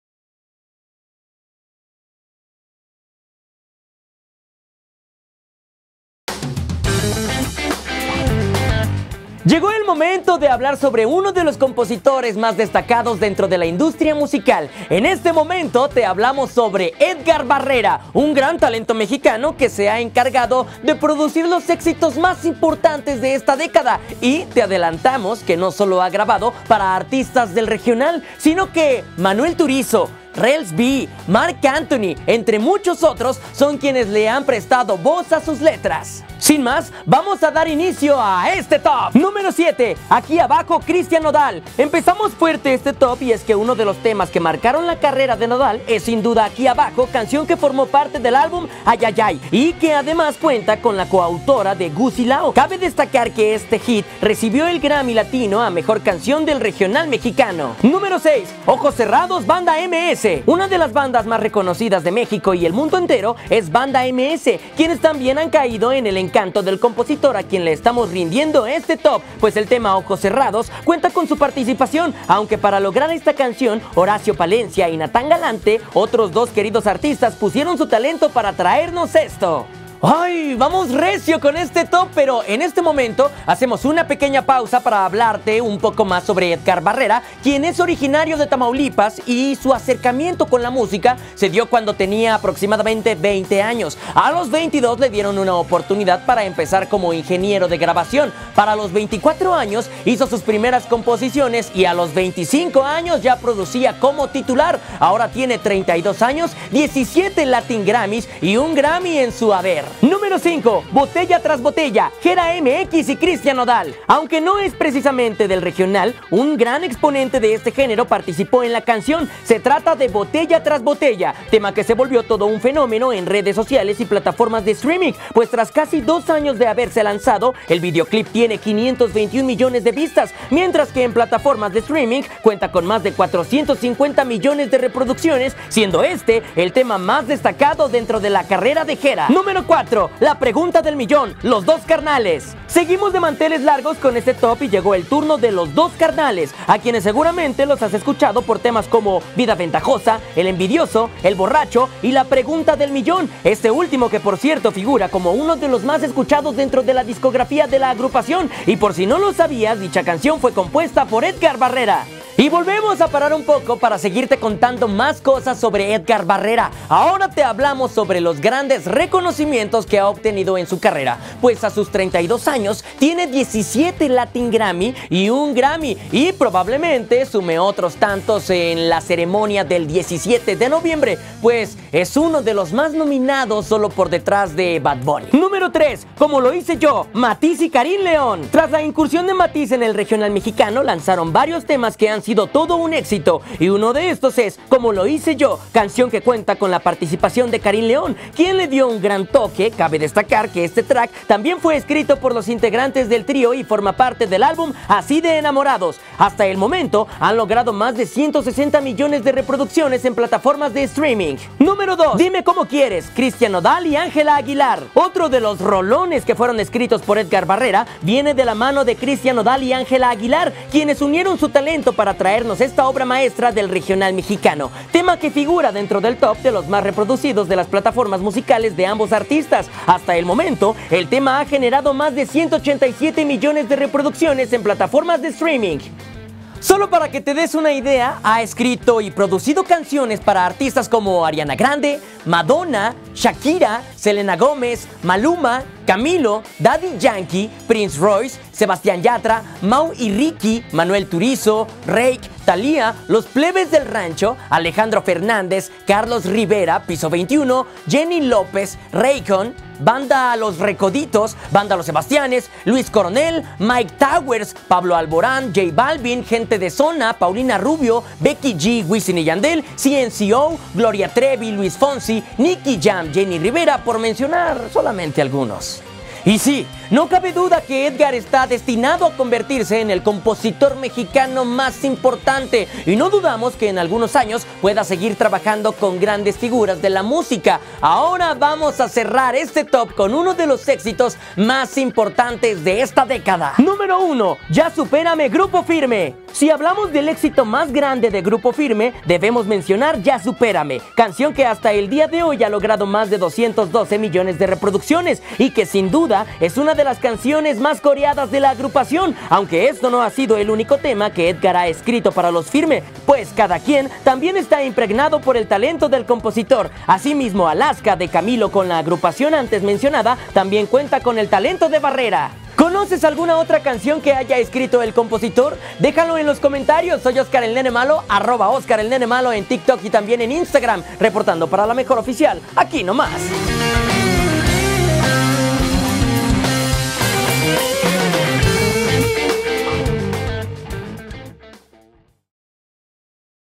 ¿Las conocías? Siete canciones que ha escrito Edgar Barrera para artistas del regional mexicano
Uno de los más grandes compositores que le ha dado muchas canciones al regional es Edgar Barrera, un hombre que gracias a su gran talento con la pluma es muy solicitado en este medio, es por esto que te traemos algunas de ellas AquíNomás 🤠